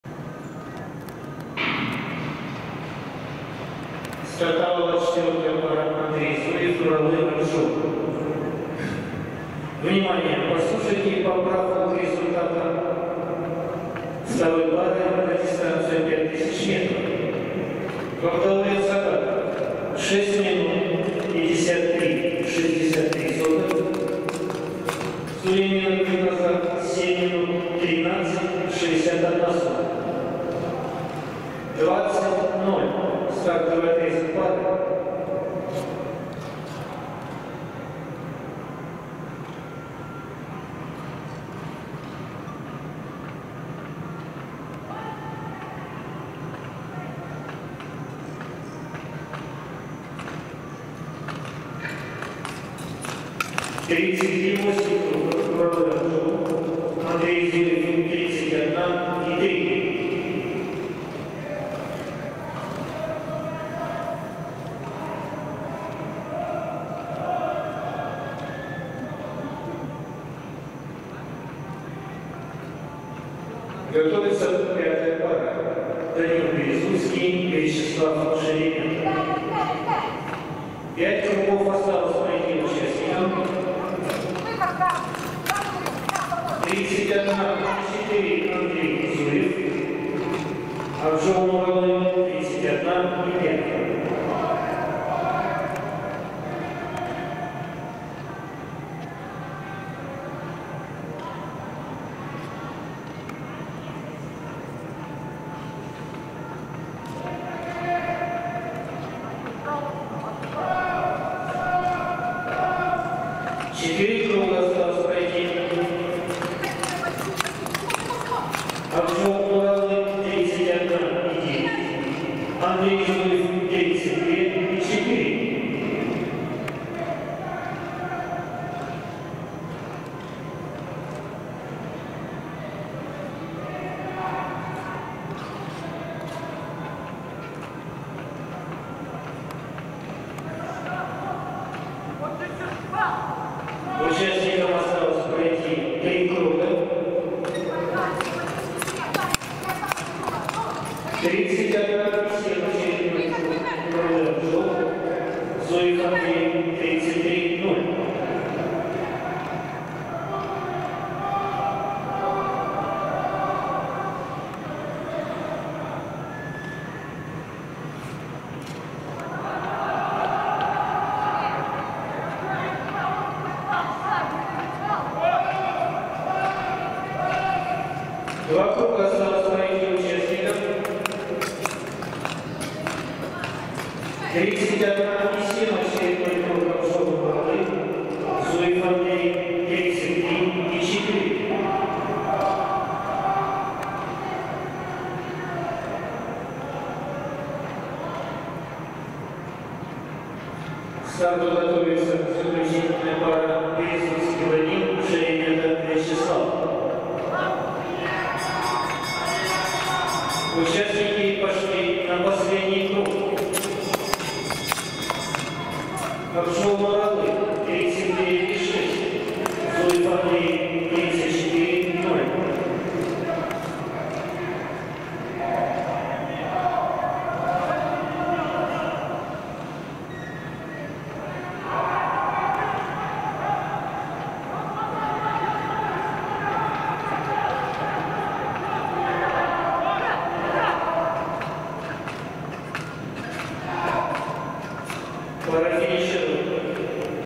Nuraly AKZHOL - Andrej ZUEV 3000M Control Training Astana (KAZ) 16 - 17 November 2020
Control Training Astana (KAZ) 16 - 17 November 2020

3000M
Nuraly AKZHOL (KAZ) - 4.04,22 PR
Andrej ZUEV (KAZ) - 4.09,32 PR

Source:  Skating.kz

Контрольная Тренировка Астана (Казахстан) 16 - 17 ноября 2020 г.

3000M
Нуралы Акжол - 4.04,22
Андрей Зуев - 4.09.32

Источник : Skating.kz